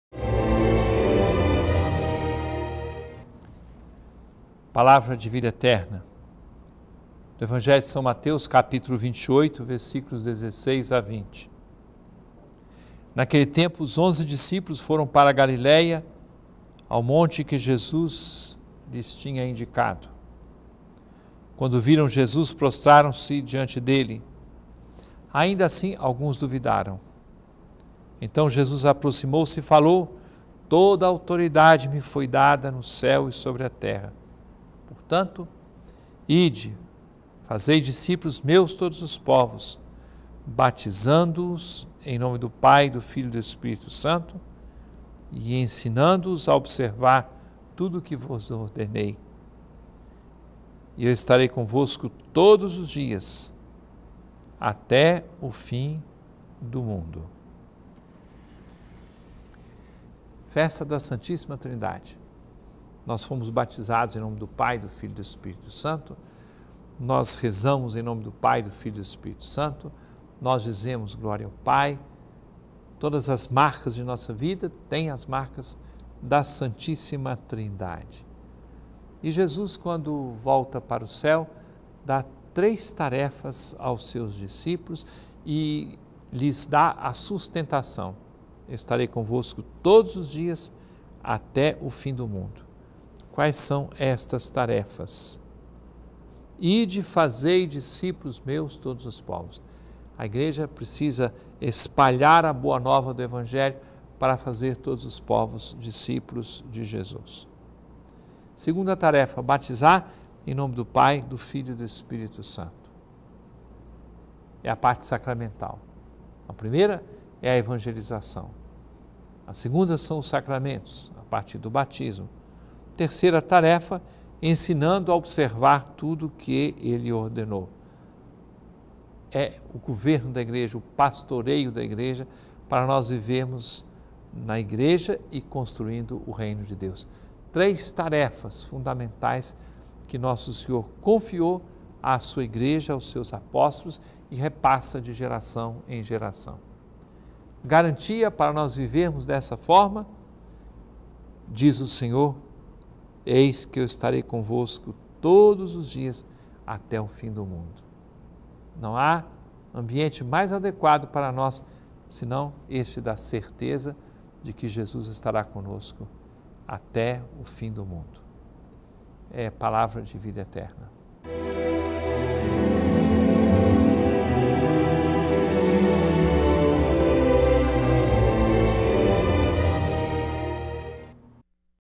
Palavra de Vida Eterna | São Mateus 28, 16-20 - 31/05/2015
Naquele tempo, 16os onze discípulos foram para a Galileia, ao monte que Jesus lhes tinha indicado.

17Quando viram Jesus, prostraram-se diante dele. Ainda assim alguns duvidaram.

18Então Jesus aproximou-se e falou: “Toda a autoridade me foi dada no céu e sobre a terra. 19Portanto, ide e fazei discípulos meus todos os povos, batizando-os em nome do Pai e do Filho e do Espírito Santo, 20e ensinando-os a observar tudo o que vos ordenei! Eis que eu estarei convosco todos os dias, até ao fim do mundo”.